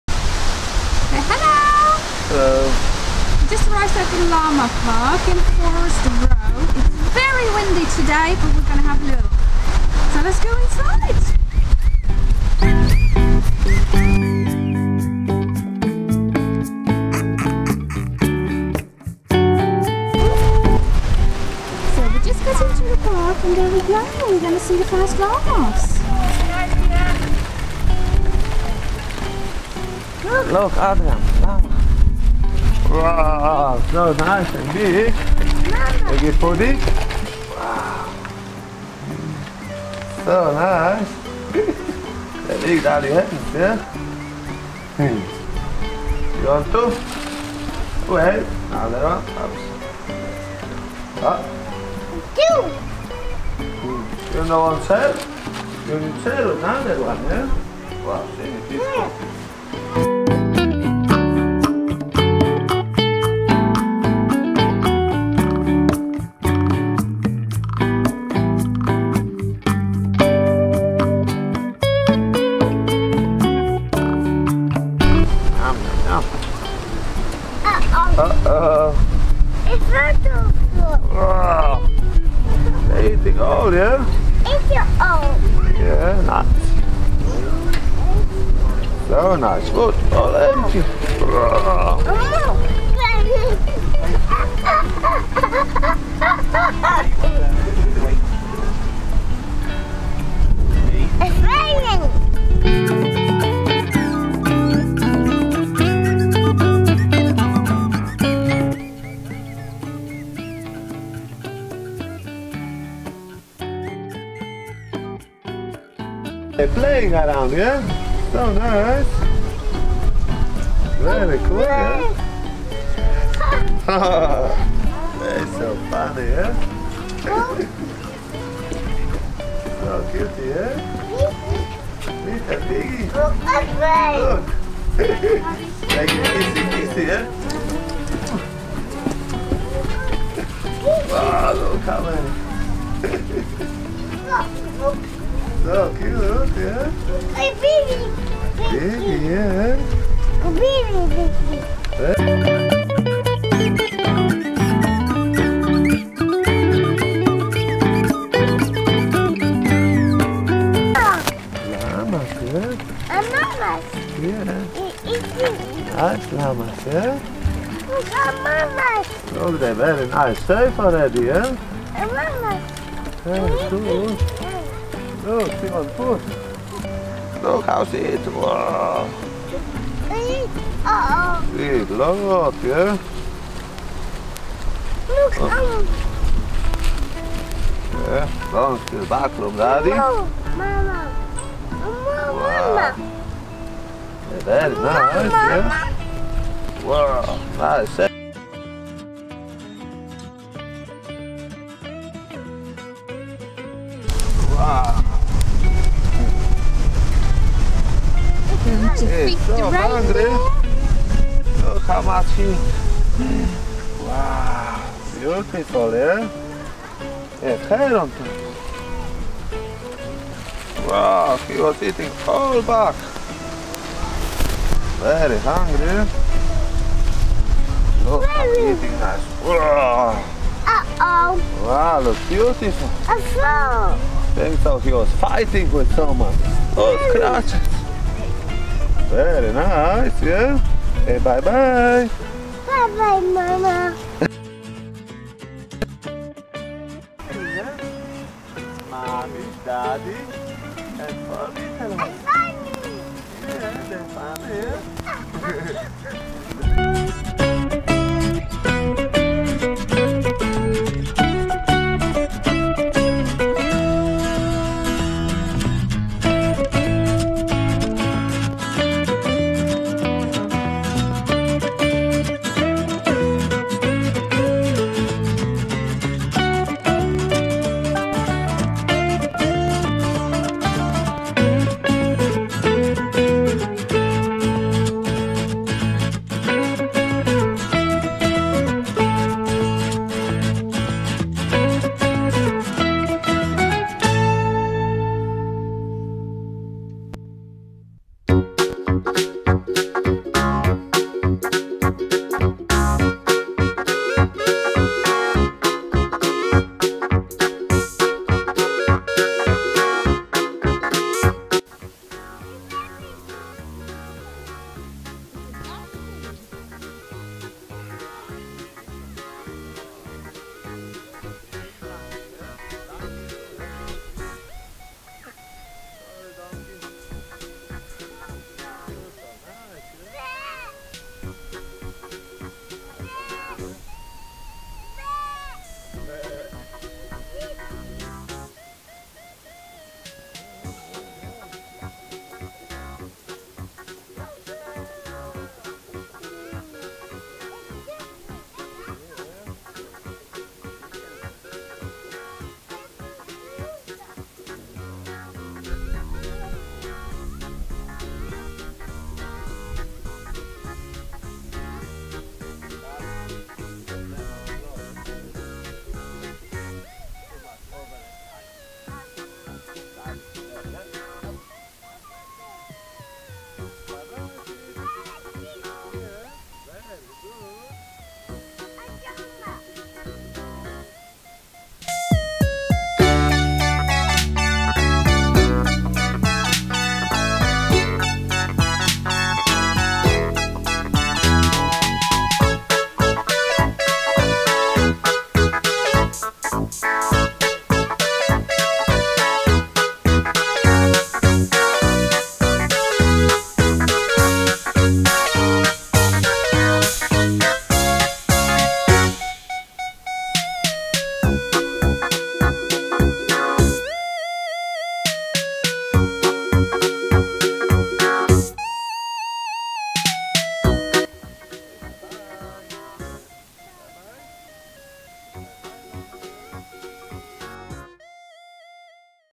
Trip To Llama Park Forest Row With Adriaan 2020
nice trip to the Llama park what Adriaan very much enjoyed. Adriaan very much enjoy's to see the animals and also learns from this the names and noises they make.